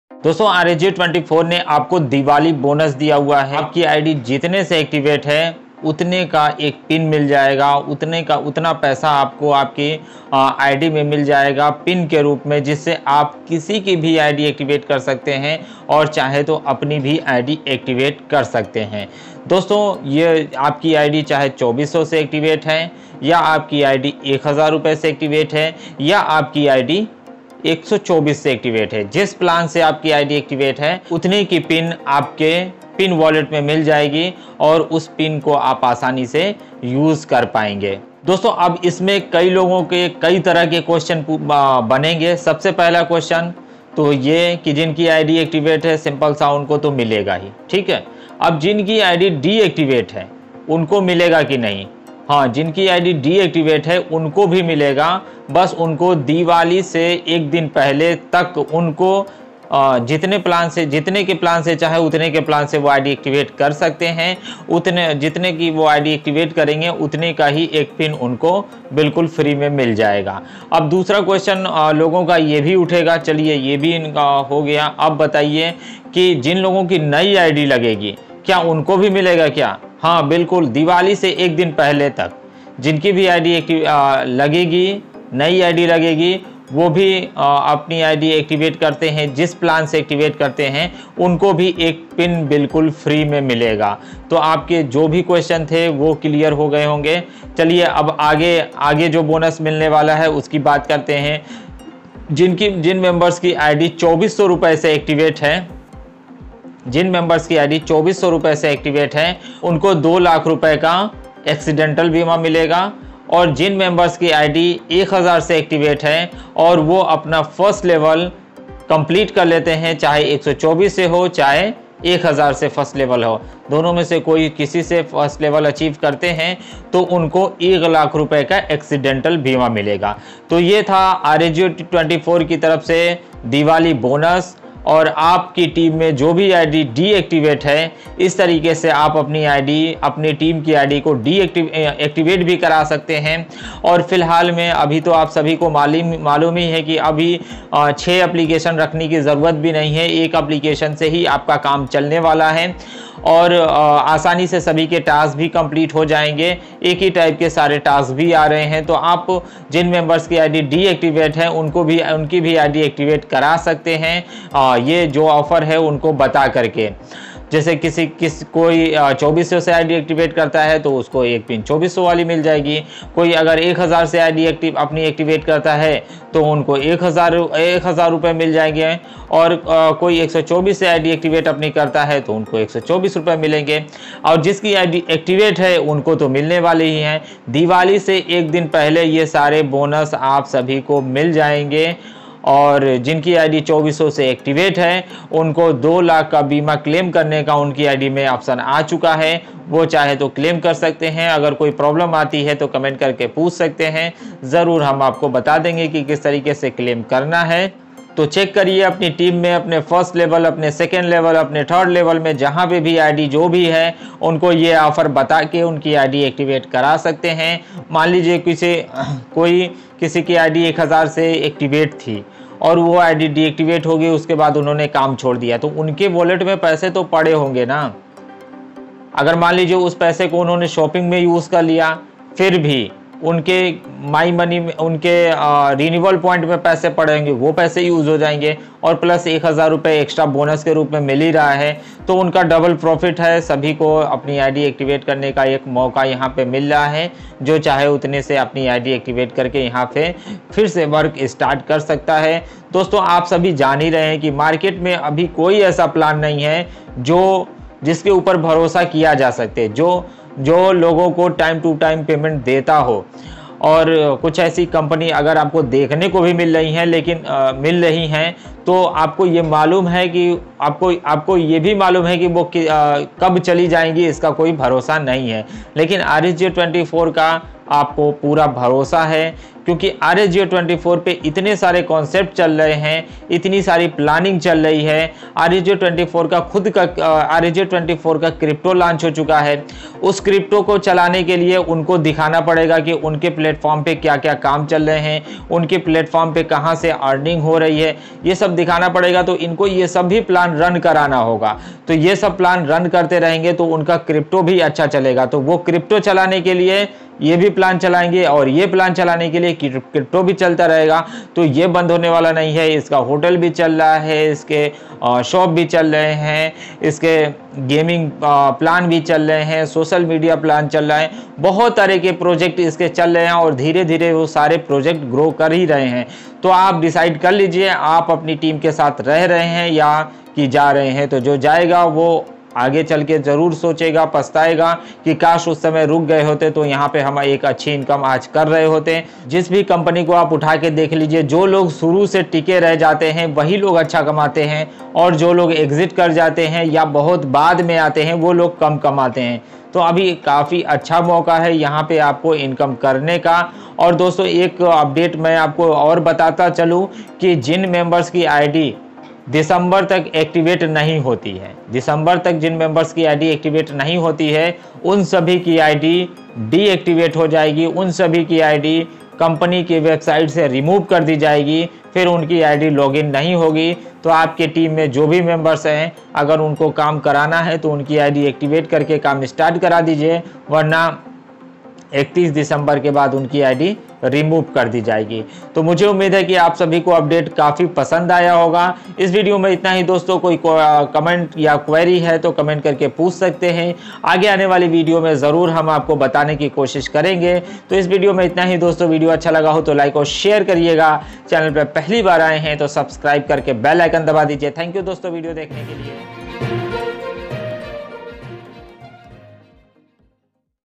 Diwali Bonus Rsgio 24 || ₹ 2400 Diwali Bonus
Dear all subscriber company aap sabhi ko diwali bonus dene jaa rahi hai kya aap diwali Bonus lene ko taiyar hain full information

rsgip 24 joinsky world joining process, rsgip 24 full joining process, how to join rsgip 24